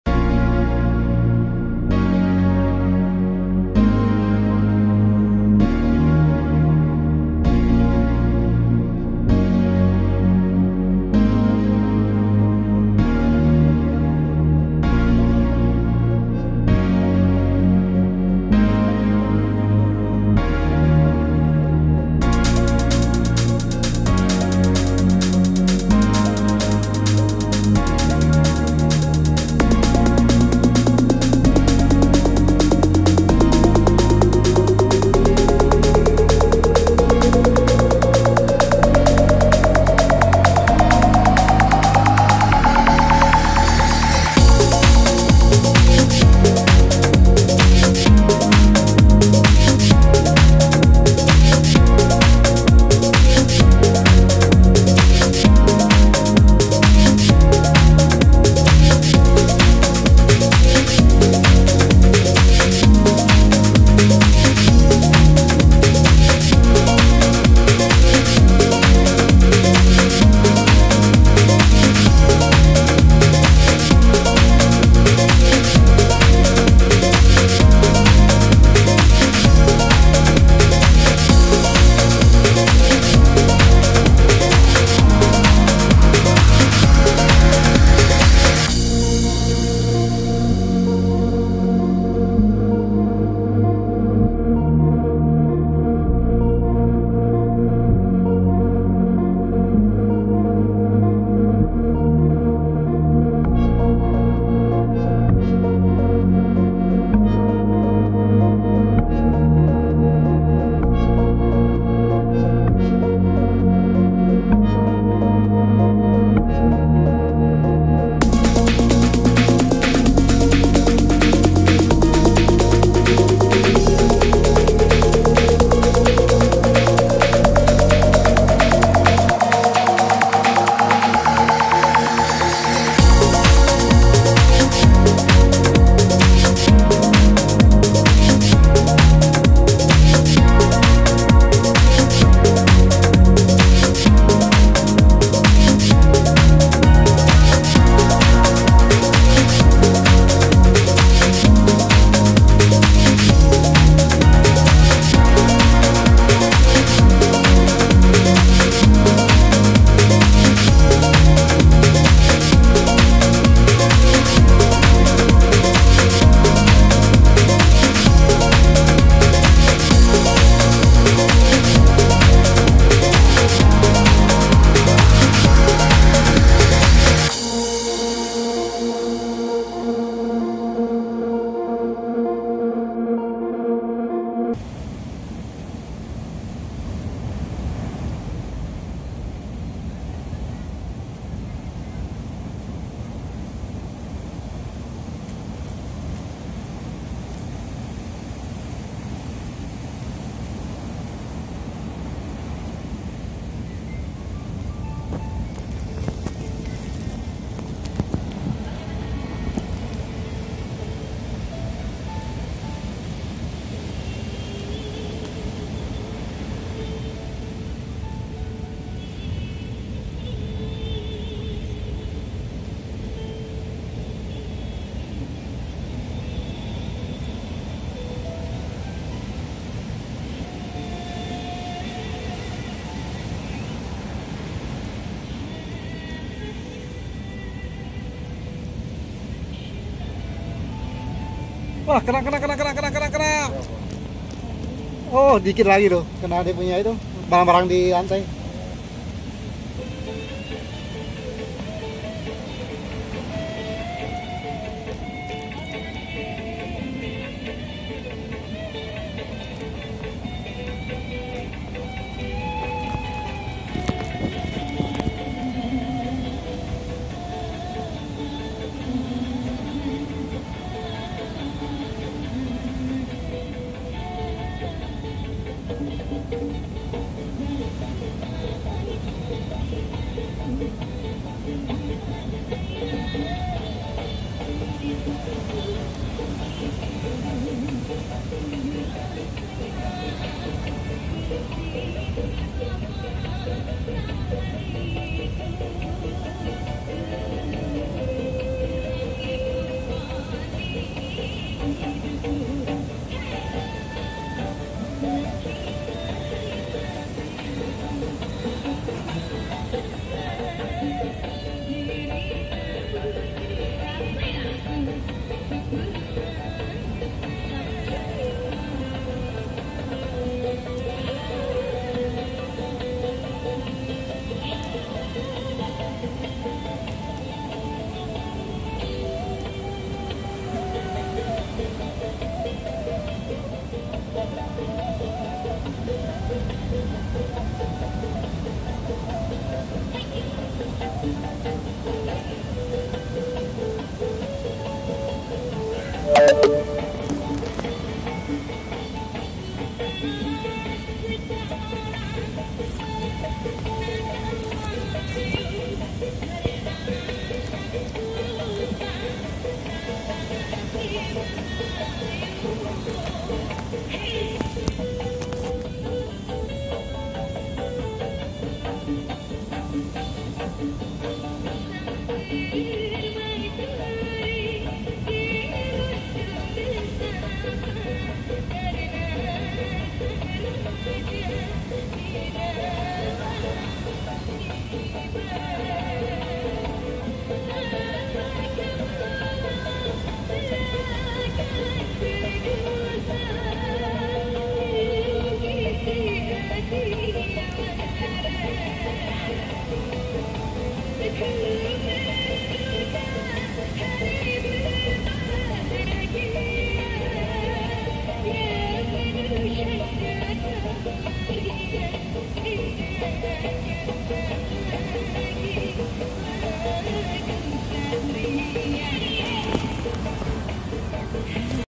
Drone Sky S167 terbang diatas Pantai Goa Seribu Candi Goa Langir
Toys Drone Sky S167 terbang di hamparan pasir putih di Goa Seribu Candi Pantai Goa Langir
OurAdventureOurTrip to Sawarna
20.1029